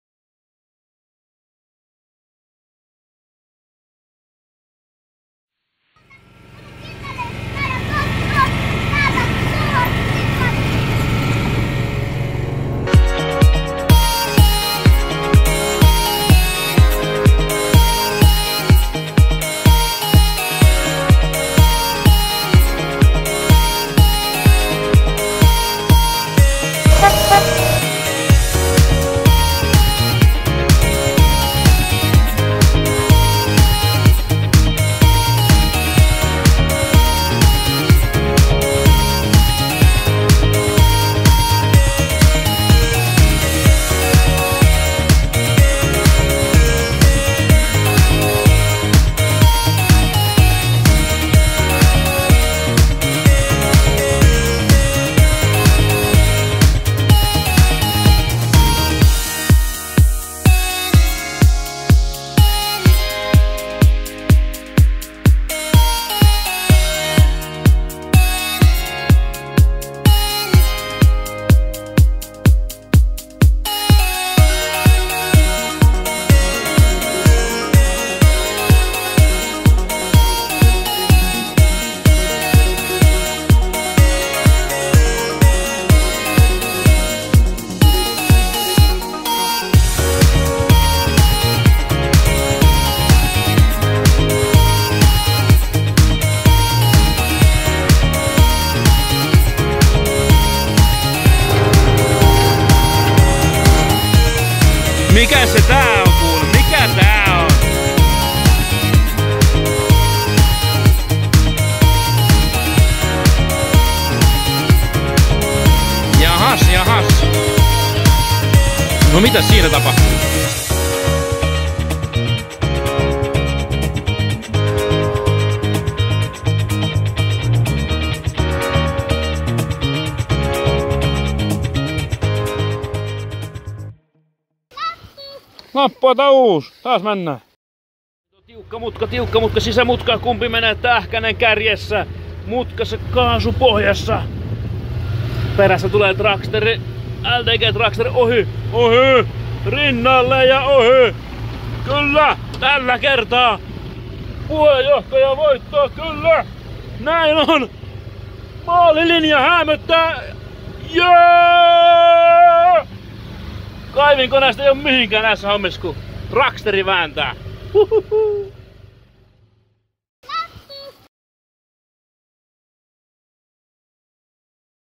NÄIN TEHTIIN HEINOLA DISCGOLFPARK WORLD – 15 VUODEN TALKOOTYÖT 2 MINUUTIN VIDEOLLA » Kippis.org
Vuosien 2005 ja 2020 välillä on talkoiltu paljon. Kertyneistä kuvista&videoista koostettiin tämä noin 3 minuutin melko intensiivinen pläjäys. Ota syvään henkeä ja laita pyörimään :)